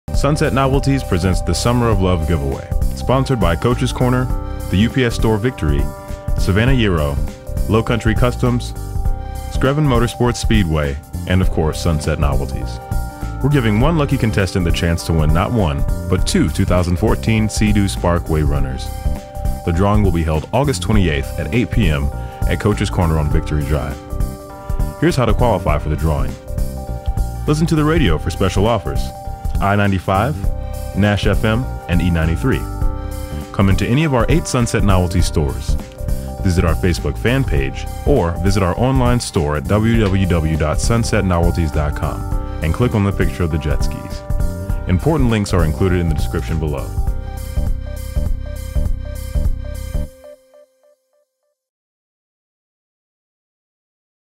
Sunset Novelties Summer of Love Giveaway
Video Highlights
00:00 Sunset Novelties Presents, the Summer of Love

00:12 Win TWO 2014 SEA-DOO Spark Waverunners

00:16 Drawing held August 28th, 8pm, Coach's Corner

00:32 Visit our Facebook Fan Page

00:37 Visit Sunset Novelties Online store

We're giving one lucky winner a chance to win not one, but TWO 2014 SEA-DOO Spark wave runners.

The drawing will be held August 28th at 8pm at Coach's Corner on Victory Drive

Here's how to qualify for the drawing:

Listen to the radio for special offers, come into any of our 9 Sunset Novelties stores, visit our Facebook Fan and click on the picture of the Jet Skis.

Links are included in the description below.